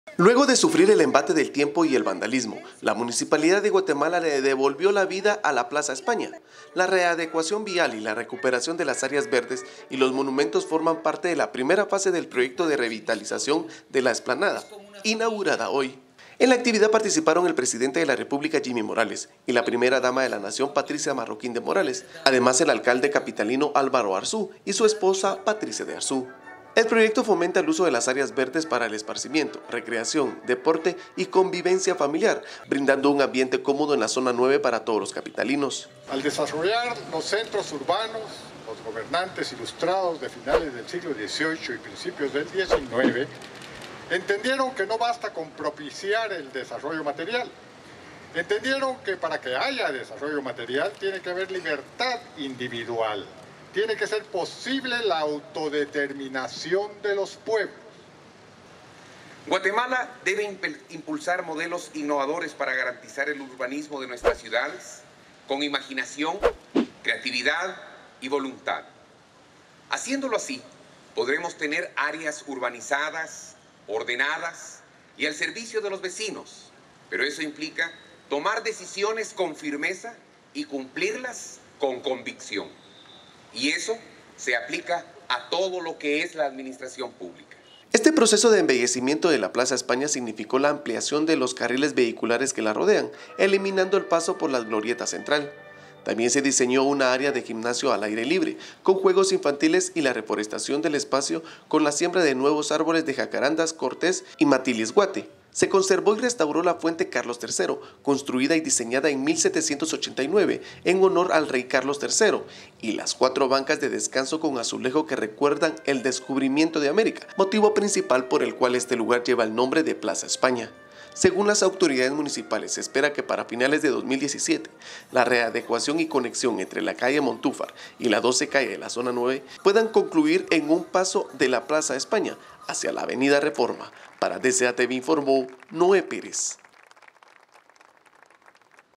Así quedó la Plaza España luego de su revitalización
Luego de sufrir el embate del tiempo y el vandalismo, la Municipalidad de Guatemala le devolvió la vida a la Plaza España.
La readecuación vial y la recuperación de las áreas verdes y los monumentos forman parte de la primera fase del proyecto de revitalización de la explanada, inaugurada hoy.

En la actividad participaron el presidente de la república Jimmy Morales y la
primera dama de la nación, Patricia Marroquín de Morales, además del alcalde capitalino, Álvaro Arzú, y su esposa Patricia de Arzú.

Se conservó y restauró la Fuente Carlos III, construida y diseñada en 1789, en honor al Rey Carlos III; y las 4 bancas de descanso con azulejo que recuerdan el descubrimiento de América, motivo principal por el que este lugar lleva el nombre de Plaza España.

Según las autoridades municipales se espera que para finales de 2017 la readecuación y conexión entre la Calle Montúfar y la 12 calle de la zona 9 puedan concluir en un paso de la Plaza España hacia la Avenida Reforma.